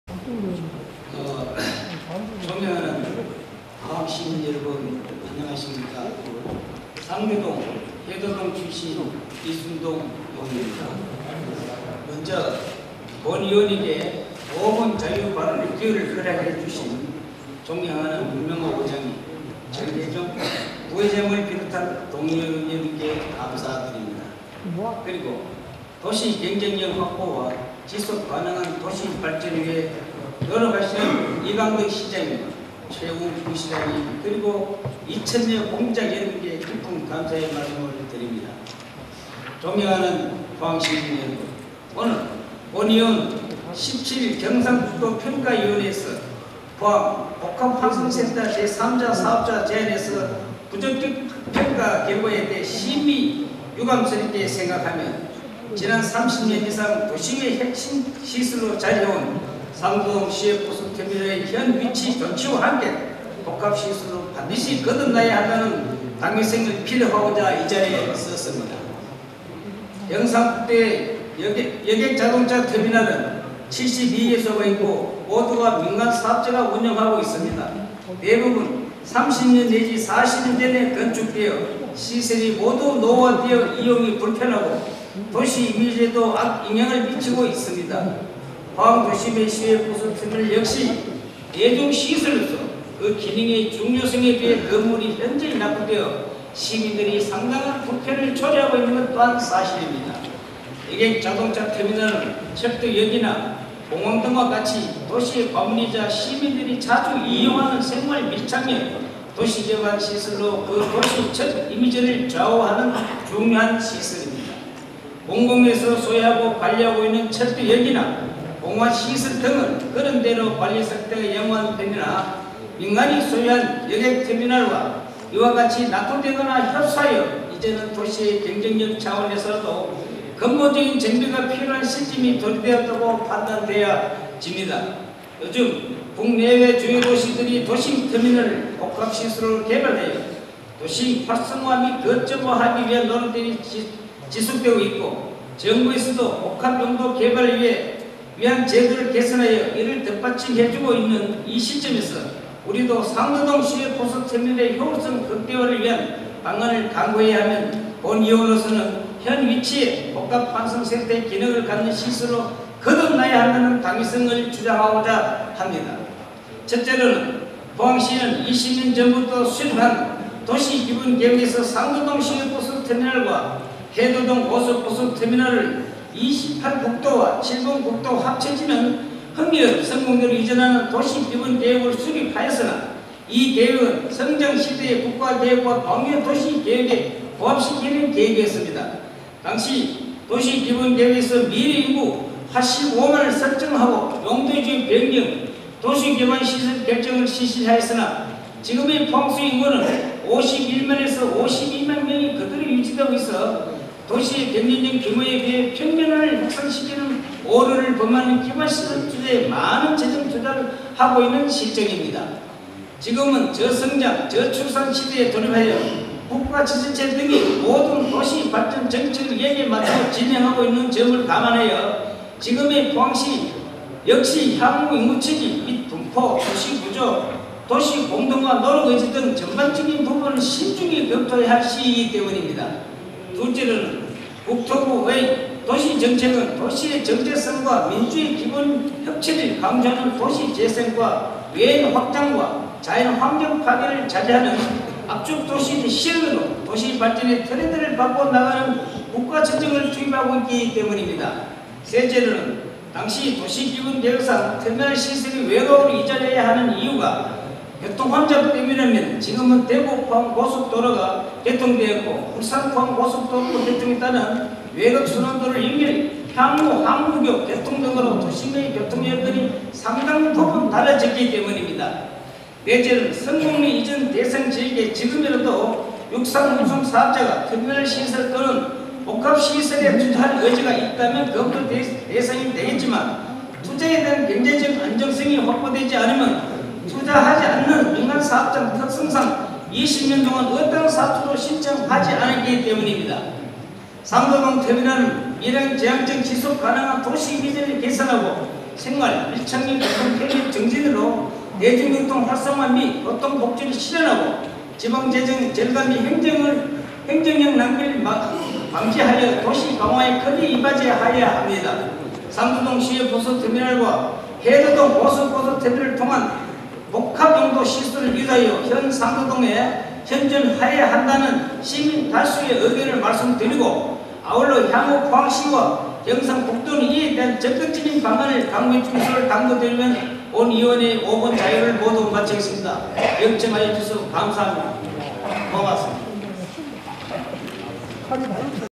제239회 포항시의회（임시회） - 이순동의원 5분자유발언
존경하는 포항시민 여러분, 안녕하십니까?
상대, 대도, 해도동 출신 이순동 의원입니다.

먼저 본 의원에게 5분 자유발언의 기회를 허락해 주신 존경하는 문명호 의장님, 정해종 부의장님을 비롯한 동료의원 여러분!
그리고, 도시 경쟁력 확보와 지속가능한 도시발전을 위해 노력하시는 이강덕 시장님과 2,000여 공직자 여러분께 깊은 감사의 말씀을 드립니다.

존경하는 포항시민 여러분!
오늘 본 의원은 지난 30년 이상 도심의 핵심시설로 자리해 온 상도동 시외버스터미널의 현 위치 존치와 함께 복합시설로 거듭나야 한다는 당위성을 피력하고자 이 자리에 섰습니다.

경상북도의 여객자동차 터미널은 72개소가 있고, 모두가 민간사업자가 운영하고 있습니다. 대부분 30〜40년 전에 건설되어 시설이 모두 노후화되어 이용이 불편하고 도시이미지에도 악 영향을 미치고 있습니다. 포항도심의 시외버스터미널 역시 대중시설로서 그 기능의 중요성에 비해 건물이 현저히 낙후되어 시민들의 상당한 불편을 초래하고 있는 것 또한 사실입니다.
여객자동차터미널은 철도역, 공항 등과 같이 도시의 관문이자 시민들이 자주 이용하는 생활밀착형 도시기반 시설로 그 도시의 첫 이미지를 좌우하는 중요한 시설입니다.

공공에서 소유하여 관리하고 있는 철도역, 공항시설 등은 그런대로 관리 상태가 양호한 편이나 민간이 소유한 여객터미널은 이와 같이 낙후되거나 협소하여 이제는 도시의 경쟁력 차원에서라도 근본적인 정비가 필요한 시점이 도래되었다고 판단되어 집니다.

요즘 국내·외 주요 도시들이 도심터미널을 복합시설로 개발하여 도심 활성화 및 거점화하기 위한 노력들이 지속되고 있고, 정부에서도 복합용도 개발을 위한 제도를 개선하여 이를 뒷받침해주고 있는 이 시점에서 우리도 상도동 시외버스터미널의 효율성 극대화를 위한 방안을 강구해야 하며 본 의원으로서는  현 위치에 복합환승센터의 기능을 갖는 시설로 거듭나야 한다는 당위성을 주장하고자 합니다.

첫째, 포항시는 20년 전부터 수립한 도시기본계획에서 상도동 시외버스터미널과 해도동 고속버스터미널을 28번 국도와 7번국도가 합쳐지는 흥해읍 성곡리로 이전하는 도시기본계획을 수립하였으나, 이 계획은 성장시대의 국가계획과 광역도시계획에 부합하는 계획이었습니다.

당시 도시기본계획에서 미래인구 85만을 설정하고 용도지역변경, 도시기반시설 결정을 실시하였으나 지금의 포항시 인구는 51〜52만 명이 그대로 유지되고 있어 도시의 경제적 규모에 비해 평면화를 확산시키는 오류를 범하여 기반시설투자에 많은 재정투자를 하고 있는 실정입니다.

지금은 저성장, 저출산 시대에 돌입하여 국가, 지자체 등이 모든 도시발전정책을 여기에 맞추어 진행하고 있는 점을 감안하여 지금의 포항시 역시 향후 인구추이 및 분포,  도시구조, 도시공동화 노력 의지 등 전반적인 부분을 신중히 감안하여야 할 시기이기 때문입니다.

둘째, 국토부의 도시정책은 도시의 정체성과 민주주의의 기본인 협치를 강조하는 도시재생과 외연확장 및 자연환경 파괴를 자제하는 압축도시의 실현으로 도시발전의 트렌드를 바꾸어 나가는데 국가재정을 투입하고 있기 때문입니다.

셋째, 당시 도시기본계획상 터미널 시설이 외곽으로 이전하여야 하는 이유가 교통혼잡 때문이라면 지금은 대구-포항 고속도로 개통, 울산-포항 고속도로로 개통에 따른 외곽순환도로 연결, 향후 항구교 개통 등으로 도심의 교통여건이 상당부분 달라졌기 때문입니다.

넷째, 성곡리 이전 대상지역에 지금이라도 육상운송 사업자가 터미널 시설 또는 복합시설(터미널, 백화점, 호텔)에 투자할 의지가 있다면 검토대상이 되겠지만 투자에 대한 경제적 안정성이 확보되지 않으면 투자하지 않는 민간사업자 특성상 20년 동안 어떠한 사업자도 신청하지 않았기 때문입니다.

다섯째, 국가나 지자체에서는 정책이나 제도를 추진하거나 사용해오다 예측하지 못한 변화가 발생하였다면 의회에 동의를 구하고 재정비해야 합니다.
지금도 5년 단위로 실시하는 도시기본계획 재정비, 20년 단위로 실시하는 교통정비 기본계획, 5년 마다 실시하는 교통정비 중기계획을 체계적으로 정비하는 과정에 있으며, 합리적 대안이 시민들의 의지표현으로 이루어지기 때문입니다.

상도동 터미널은 미래지향적, 지속가능한 도시 이미지를 개선하고 생활밀착형 교통편의 증진으로 대중교통 활성화 및 교통복지를 실현하고 지방재정 절감 및 행정력 낭비를 방지하여 도시강화에 크게 이바지하여야 합니다.

상도동 시외버스터미널과 해도동 고속버스터미널을 통합, 복합용도 시설로 유도하여 현 상도동에 현존하여야 한다는 시민 다수의 의견을 말씀드리며, 본 의원의 5분 자유발언을 모두 마치겠습니다.

경청해 주셔서 감사합니다.